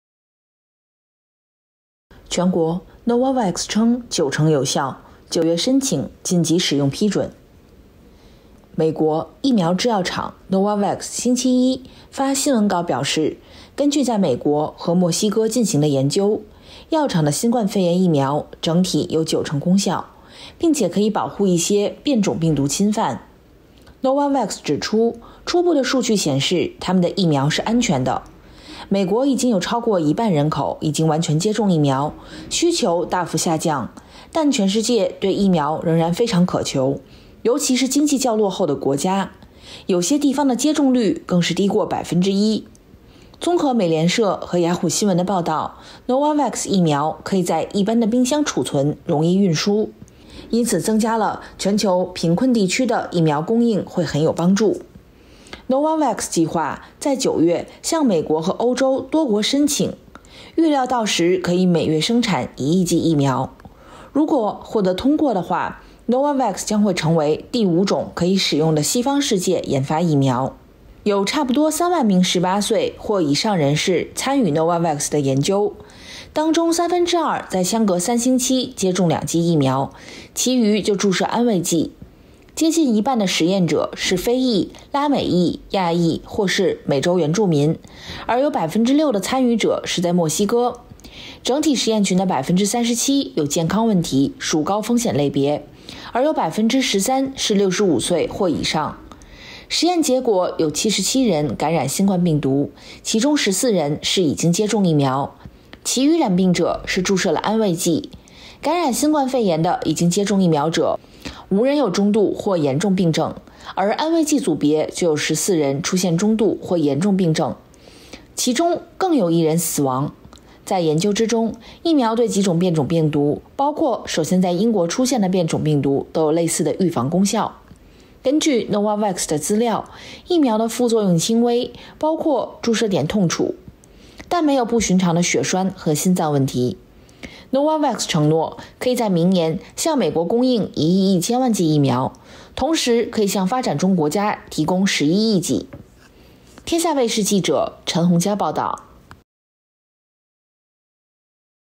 【天下新聞】全國: Novavax 稱九成有效 9 月申請緊急使用批准 Sky Link TV Chinese News 06142021
【天下新聞】Novavax claims 90% will be valid in September to apply for emergency use approval

美國疫苗製藥廠 Novavax 星期一發新聞稿表示
根據在美國和墨西哥進行的研究
藥廠的新冠肺炎疫苗整體有九成功效
並且可以保護一些變種病毒侵犯
Novavax 指出初步數據顯示
他們的疫苗是安全的
美國已經有超過一半人口已經完全接種疫苗
需求大幅下降
但全世界對疫苗仍然非常渴求
尤其是經濟較落後國家
有些地方的接種率更是低過 1%

綜合美聯社和雅虎新聞的報道
Novavax 疫苗可以在一般的冰箱儲存
容易運送
因此增加全球貧窮地區的疫苗供應會很有幫助

Novavax 計劃在 9 月
向美國和歐洲多國申請
預料到時可以每月生產 1 億劑疫苗
如果獲得通過的話
Novavax 將會成爲
第五種可以使用的西方世界研發疫苗
Pfizer、Moderna、
和 Johnson & Johnson 已經在美國可以使用
而歐洲更同時批准使用 AstraZeneca

有差不多三萬名 18 嵗
或以上人士參與 Novavax 的研究
當中三分之二在相隔三個星期接種兩劑疫苗
其餘就注射安慰劑
接近一半實驗者是非洲裔、拉美裔、亞裔或是美洲原住民
而有6%參與者是在墨西哥。
整體試驗群的 37 % 有健康問題
屬高風險類別
而有 13%是 65 嵗或以上

實驗結果有 77 人感染新冠病毒
其中 14 人是已經接種疫苗
其餘染病者是注射了安慰劑
感染新冠肺炎的已經接種疫苗者
無人有中度或嚴重病症
而安慰劑組別就有 14 人出現中度或嚴重病症
其中更有一人死亡
在研究之中
疫苗對幾種變種病毒
包括首先在英國出現的變種病毒
都有類似的預防功效
根據 Novavax 的資料
疫苗的副作用輕微
包括注射點痛楚
但沒有不尋常的血栓和心臟問題
Novavax 承諾可以在明年
向美國供應 1.1 億劑疫苗
同時可以向發展中國家提供 11 億劑